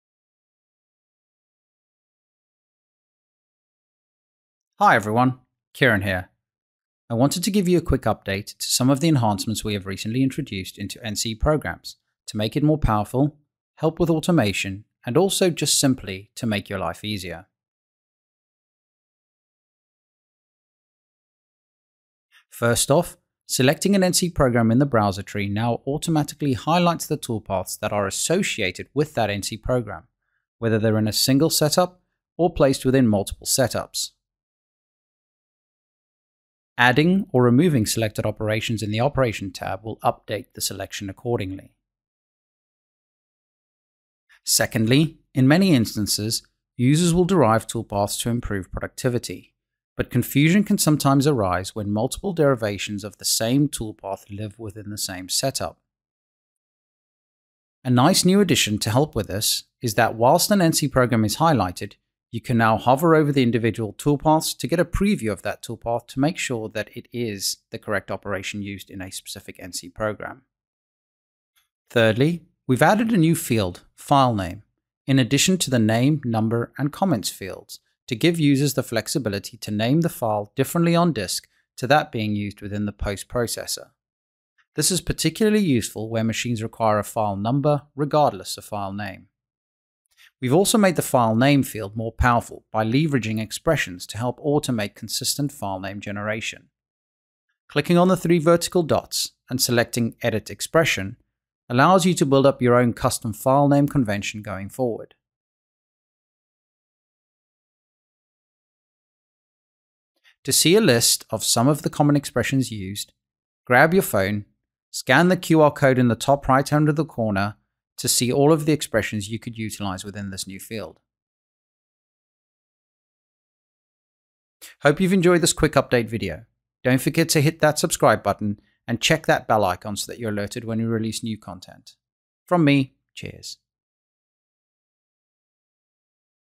NC Program Updates | Autodesk Fusion 360
In this Autodesk Fusion 360 YouTube video, Kieran will describe the new NC programs enhancements that will add power, help with automation, and make your life easier.

First, selecting an NC program in the browser tree now automatically highlights the toolpaths that are associated with that NC program, whether they’re in a single setup or placed within multiple setups. Adding or removing selected operations in the operation tab will update the selection accordingly.

Second, in many instances, users will derive toolpaths to improve productivity, but confusion can sometimes arise when multiple derivations of the same toolpath live within the same setup. Now, while an NC program is highlighted, you can hover over individual toolpaths to get a preview of that toolpath and make sure it’s the correct operation used in a specific NC program.

Third, Fusion 360 has added a new field “File name” in addition to the Name/Number and Comments fields. This new field gives users the flexibility to name the file differently on disk from the name being used within the post processor. This is particularly useful when machines require a file number regardless of file name. Fusion 360 is also leveraging expressions to help automate consistent file name generation, making the “File Name” field more powerful. By clicking the 3 vertical dots and selecting “Edit Expression,” you can build a custom File Name convention going forward. To see a list of some of the common expressions used, go